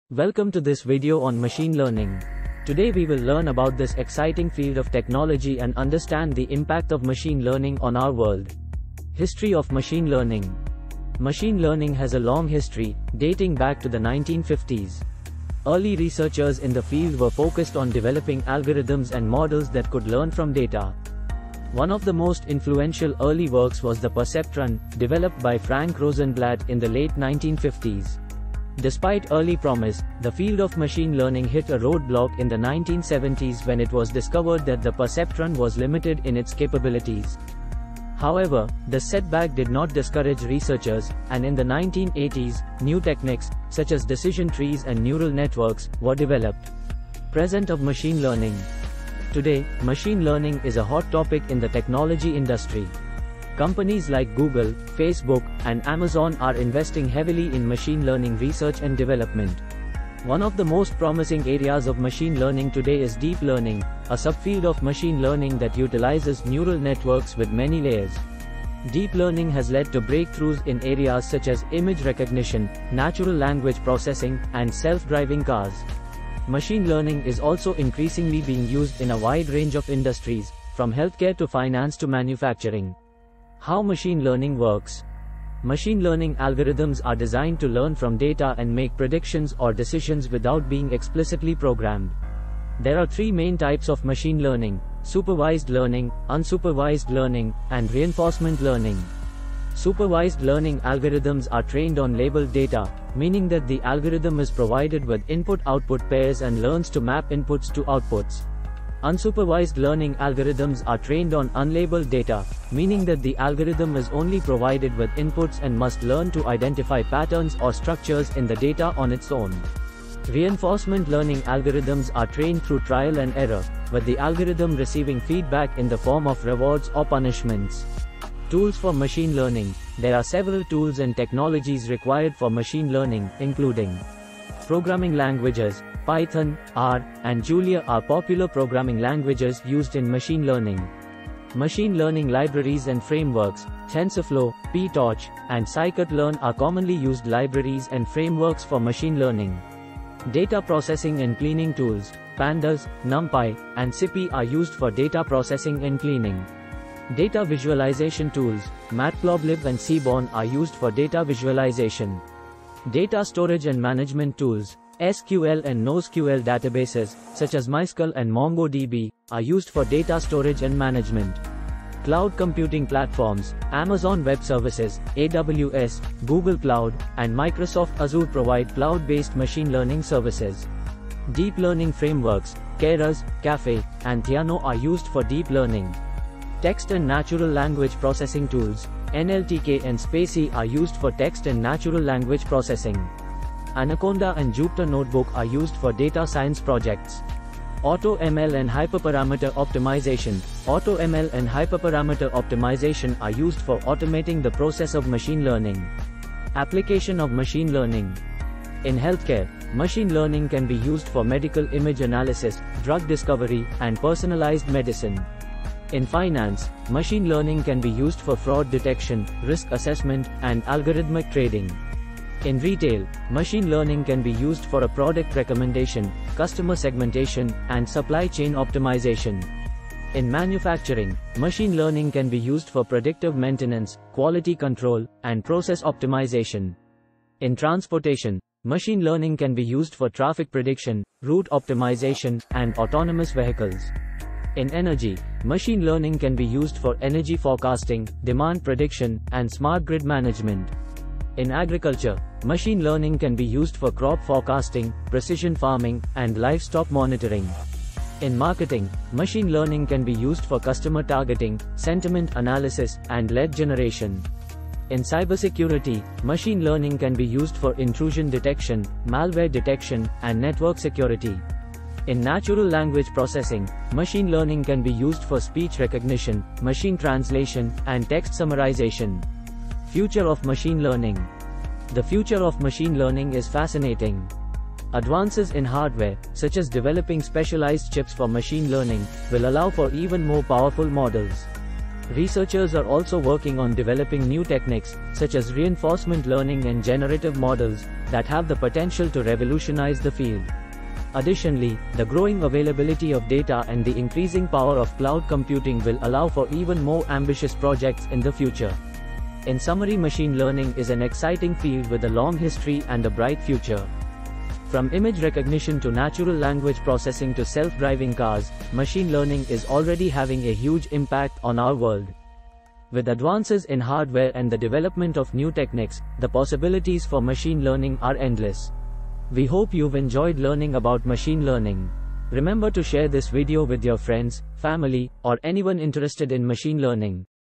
Machine Learning: What You Need to Know, How It's Impacting Our Lives, and Applications
In this video, I will discuss machine learning, what it is, how it's impacting our lives and some of its applications. If you're at all interested in this topic, then this video is for you!
science made easy by simple description for all age learners.

Best Sellers in Computers & Accessories
Best Sellers in Software
Best iPhone11 Tempered Glass